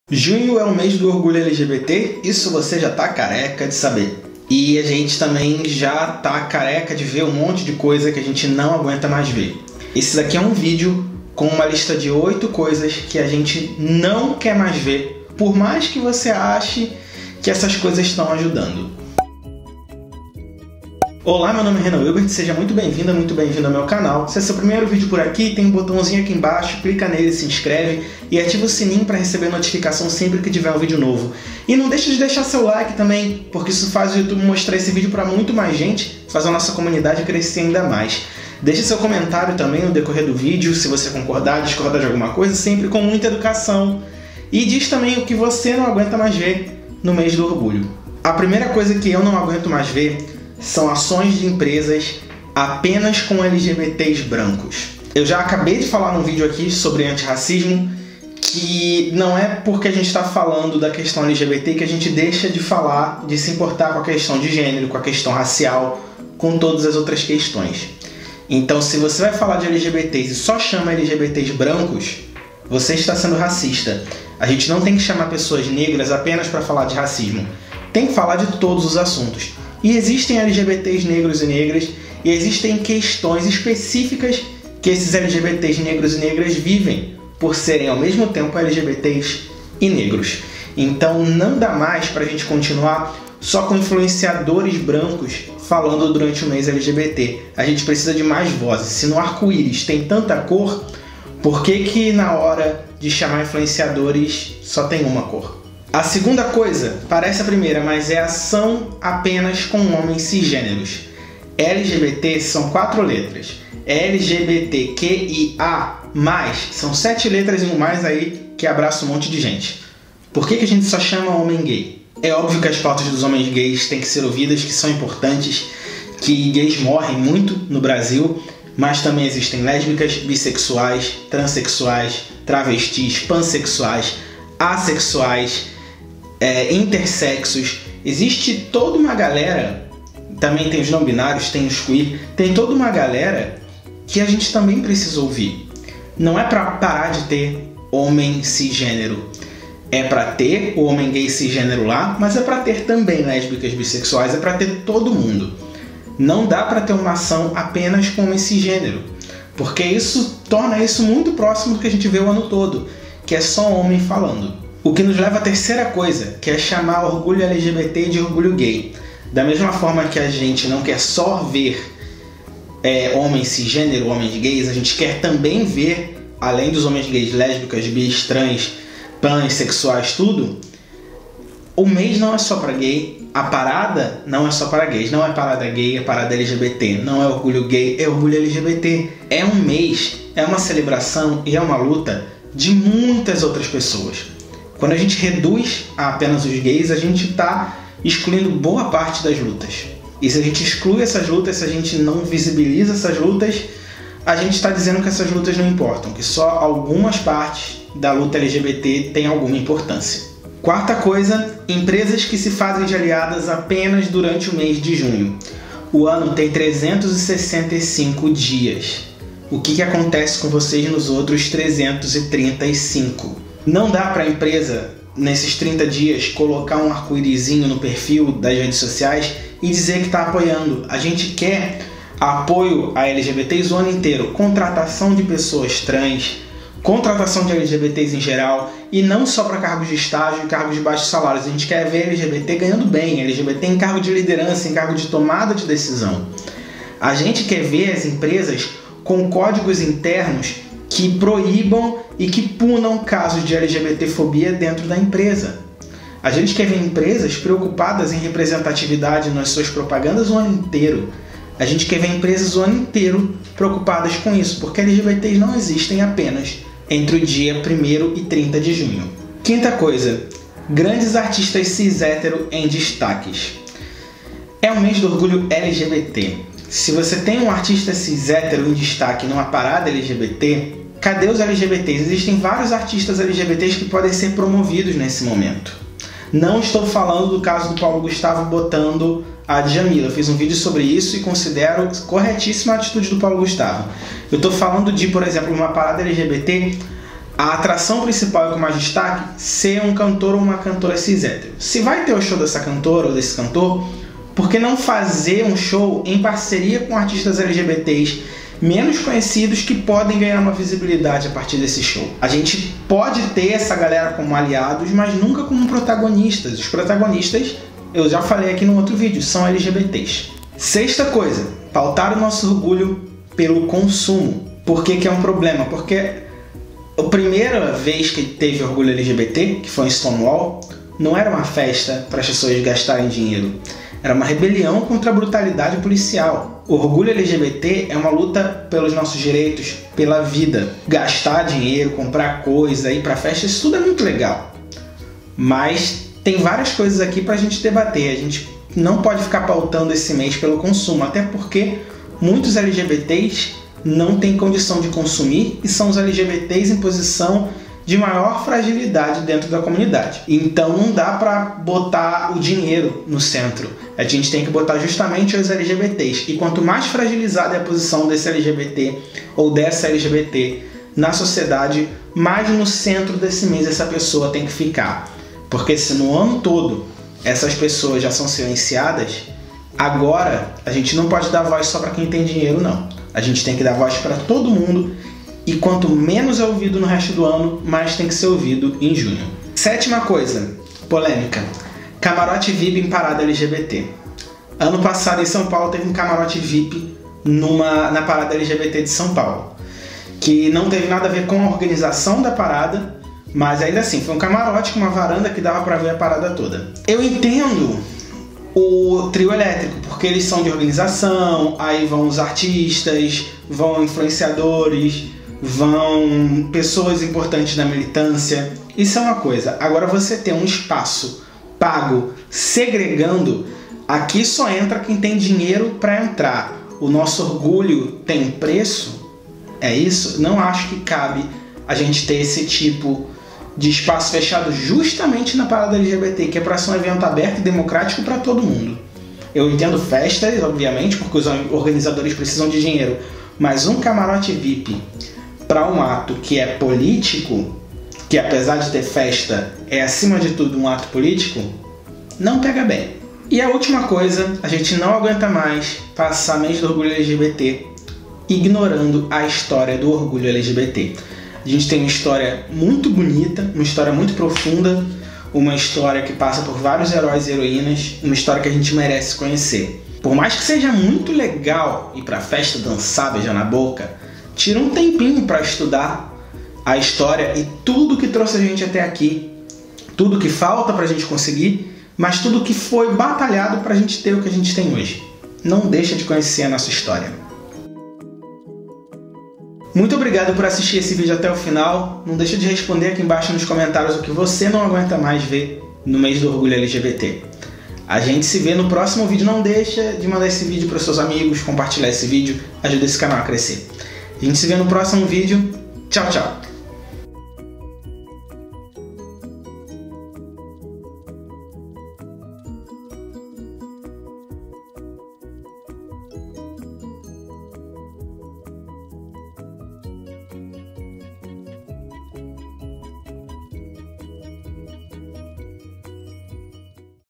8 coisas que NÃO QUEREMOS no mês do ORGULHO LGBT | Renan Wilbert
Algumas coisas no mês do Orgulho LGBT, que é comemorado sempre em junho, são feitas até com boas intenções, mas não são aquilo que a gente quer nem para o mês do  orgulho e nem para a comunidade LGBT.  Neste vídeo, cito 8 dessas coisas e explico por que não as queremos mais.